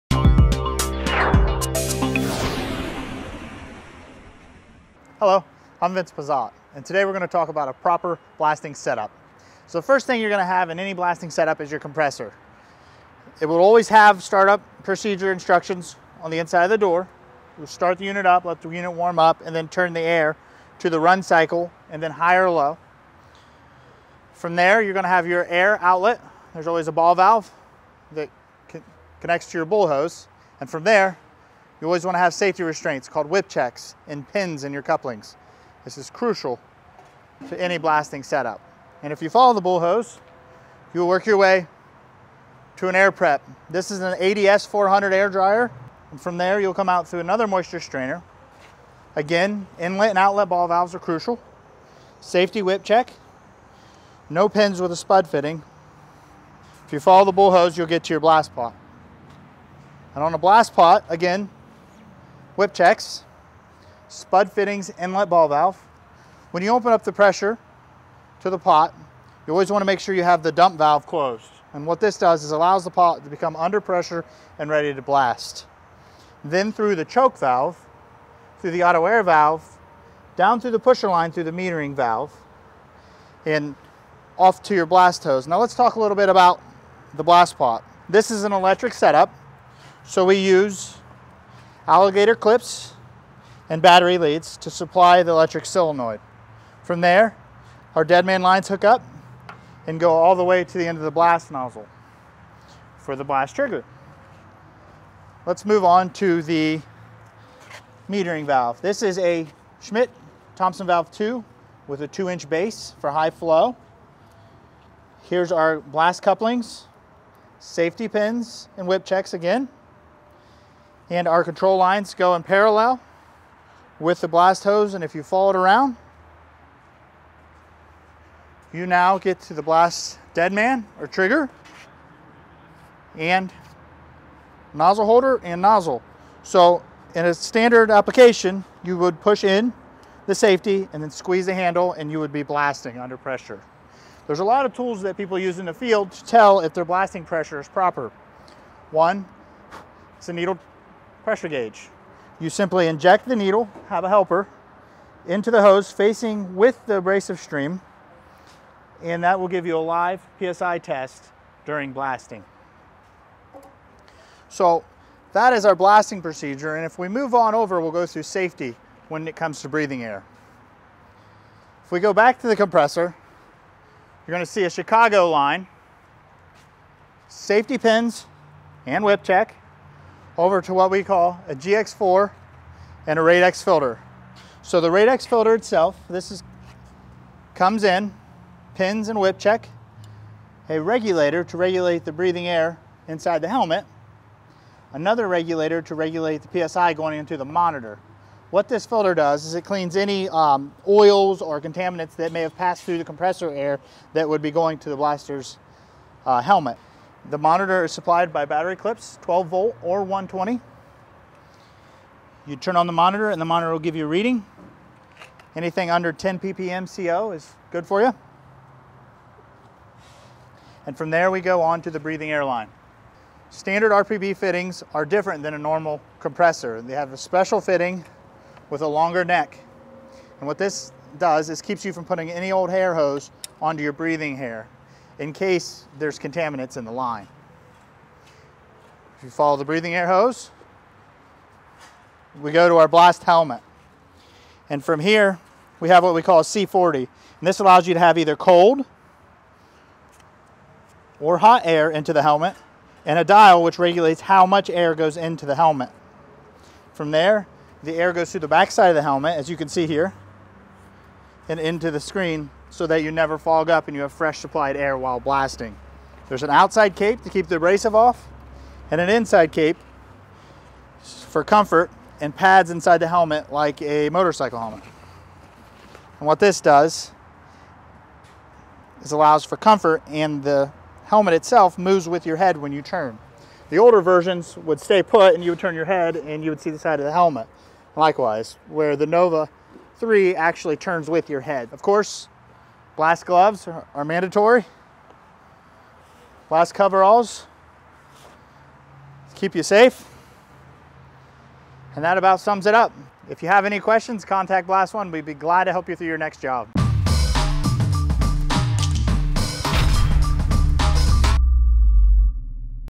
Sand blast set up 101 - easy how to guide (for beginners)
In this Primed Insight, Vince gives us a thorough overview of how to set-up your blast operation for maximum safety and productivity.
This video includes:
• Most important start up and operational steps
• What hose goes where
• A few safety pointers
• Summary of all the pieces you need when renting or buying a blast machine for the first time.